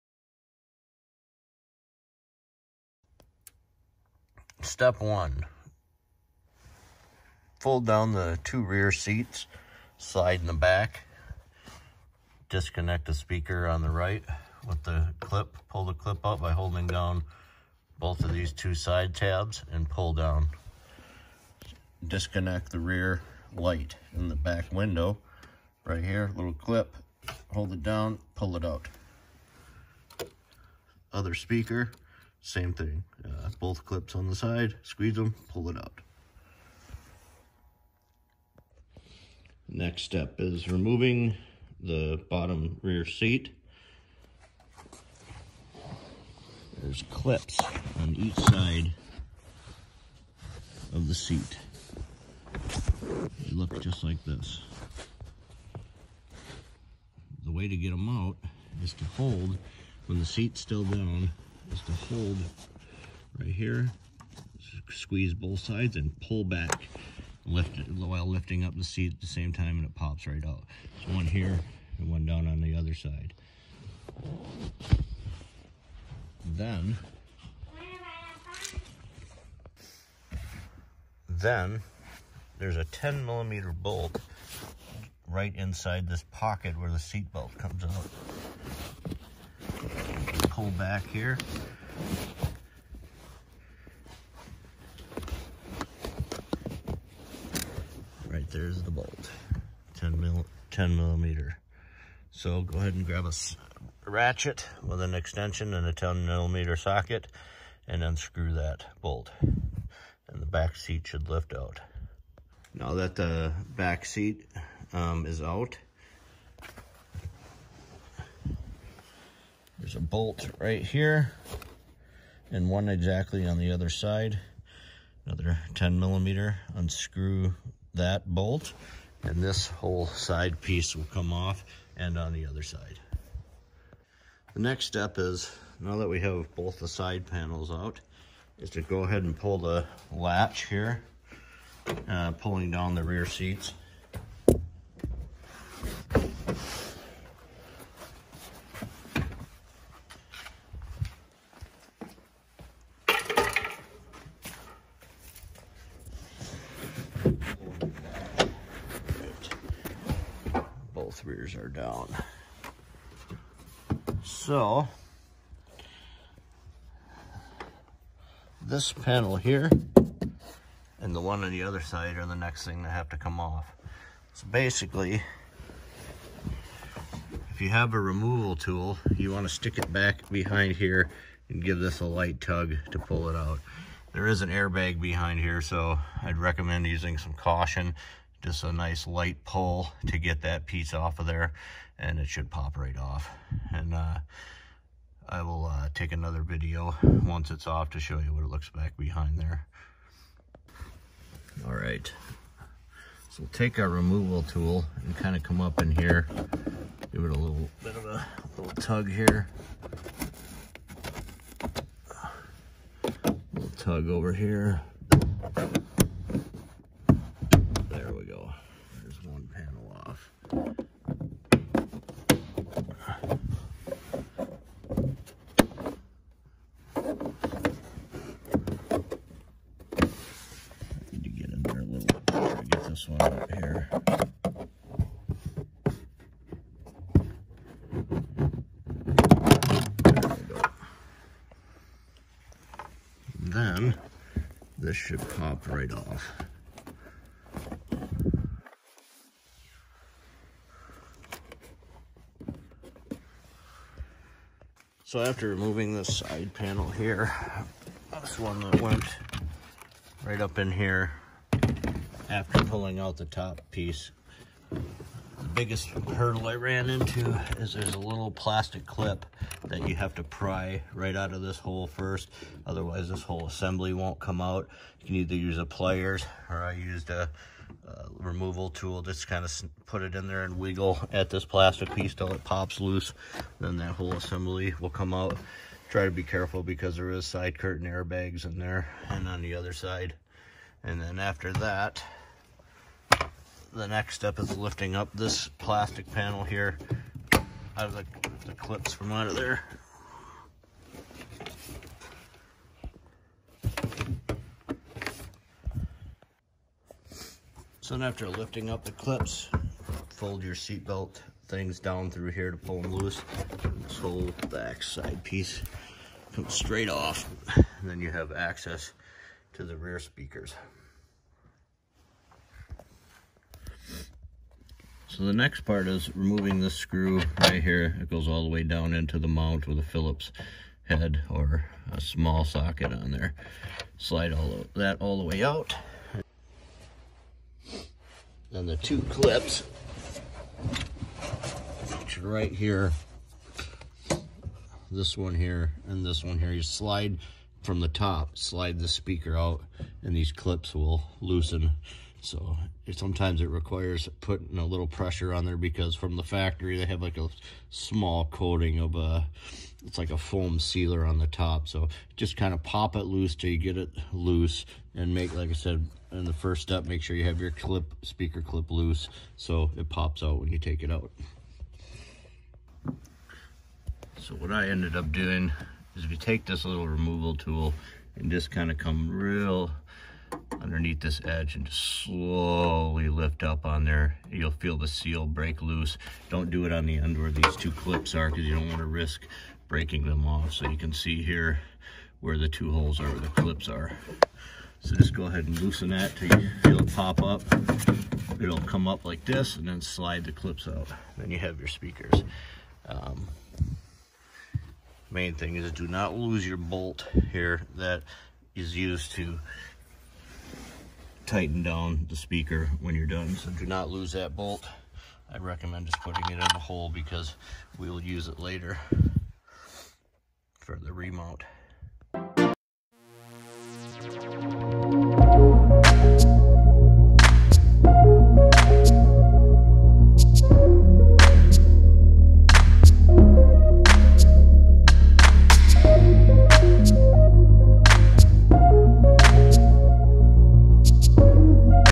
2015 Honda Civic EX Rear Speaker Removal
This video is a DIY with step by step instructions for removing the rear speakers in a 2015 Honda Civic EX Sedan.

(I have another video on my channel that explains in detail how to remove the factoty speakers from the mounts, and install a new speaker in the existing factory mounting bracket.) That video can be found here.